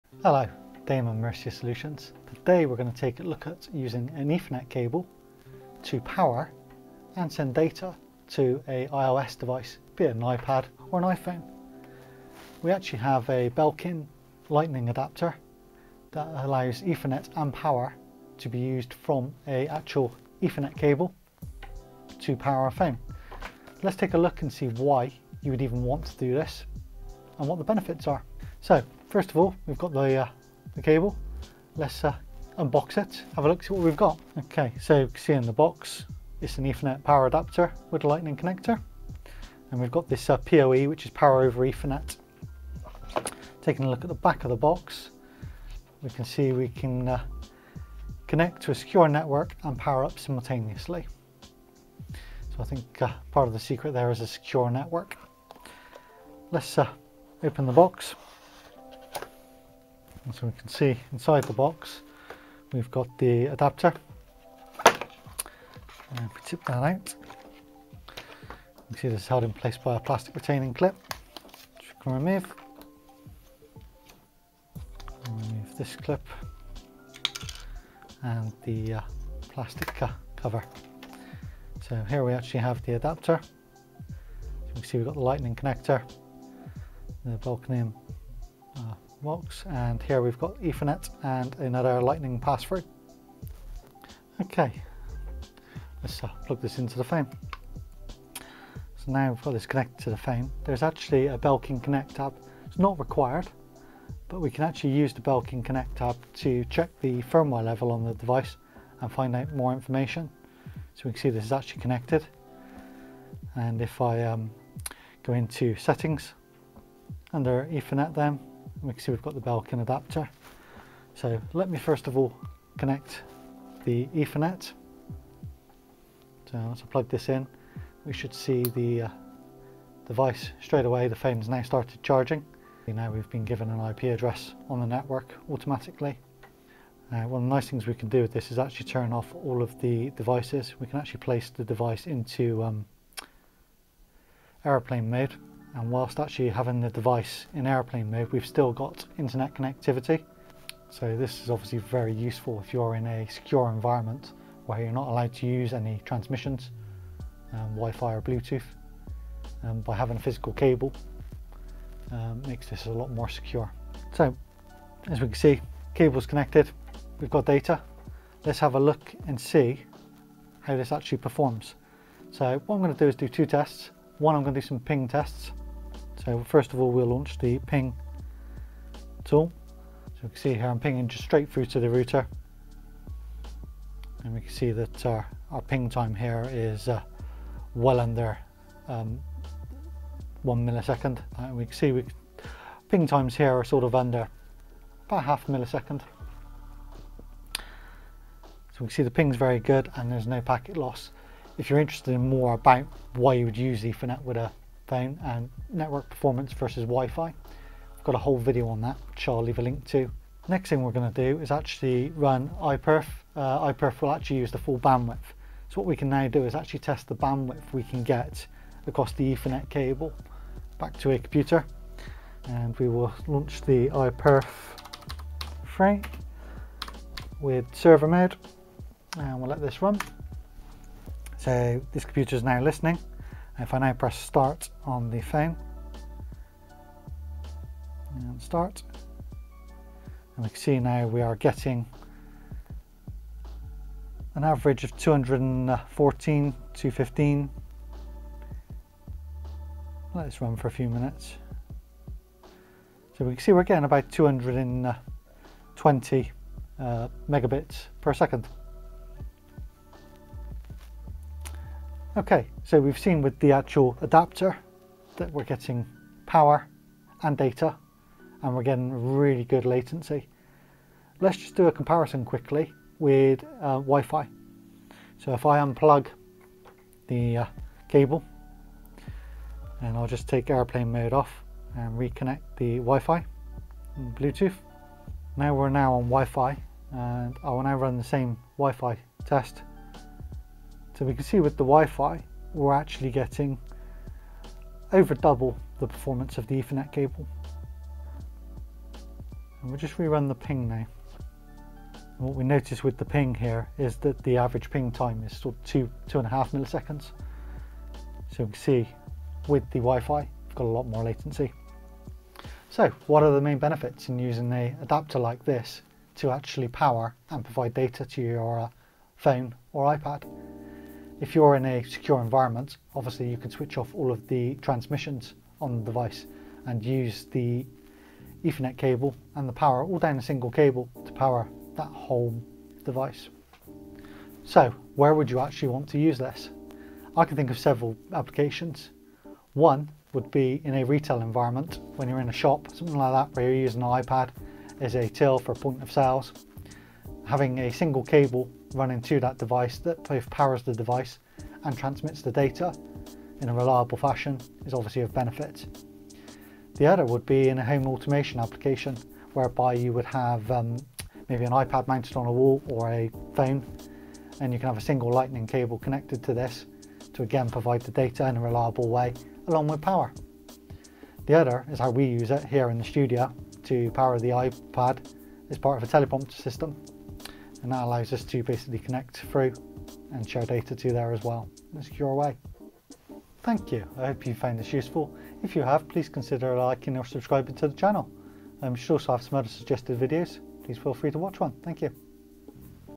Why use Ethernet to charge your iPhone?
We look at the Belkin Ethernet + Power Adapter with Lightning Connector from Belkin.

We unbox the adapter and show how it charges and the benefits of using ethernet with an ios device.

We test and compare the latency and bandwidth with ping tool and iperf3

0:00 Intro
0:34 Unboxing
1:41 Belkin connect app
2:20 Airplane mode with ethernet
2:59 Ping test latency
3:40 Iperf3 test
4:30 WiFI comparisons
5:12 Benefits